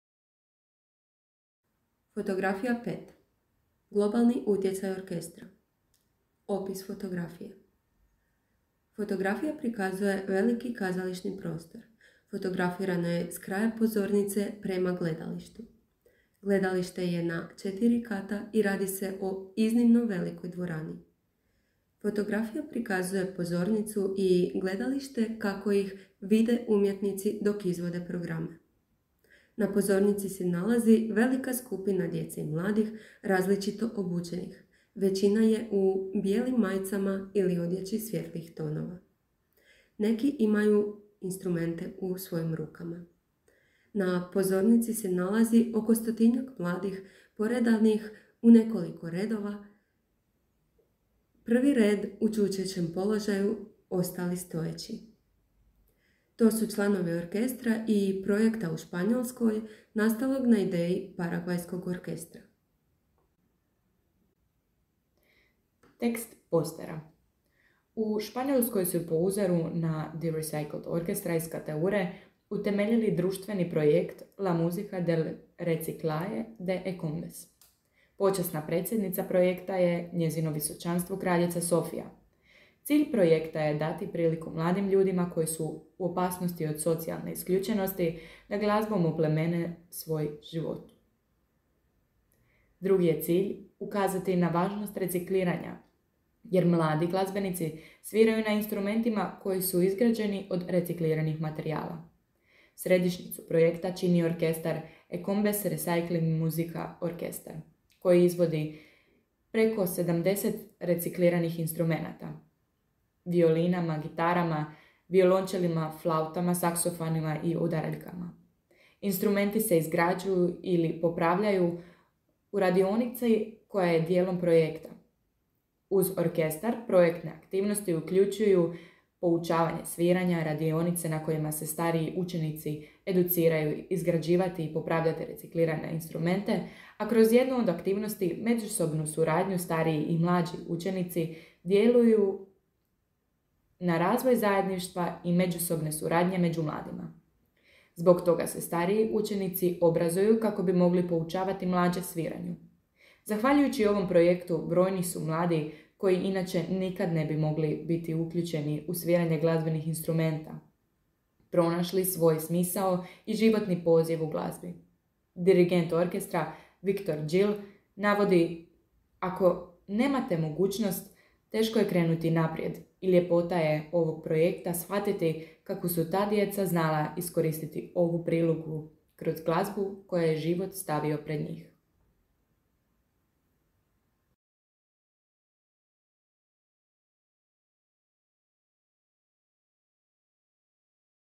"Otpad koji postaje glazba" - Zvučni zapis inkluzivnog postera - F 5 - GLOBALNI UTJECAJ ORKESTRA
ISC GREEN, Osijek, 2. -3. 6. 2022.
Video za potrebe inkulzivnog postera za Međunarodnu studentsku konferenciju Green, snimljeni su video zapisi cijelog postera i podijeljeni na 6 dijelova:
1. Uvod i sažetak
2. Paragvaj
3. Cateura
4. Reciklirani orkestar iz Cateure
5. Građa instrumenata
6. Globalni utjecaj orkestra
Izradile:
Dunja Keža, studentica 4. godine preddiplomskog sveučilišnog studija Glazbena pedagogija
Lana Čeko, studentica 2. godine preddiplomskog sveučilišnog studija Pjevanje
Mentor: dr.sc. Mirna Sabljar, v.pred. Odsjek za glazbenu umjetnost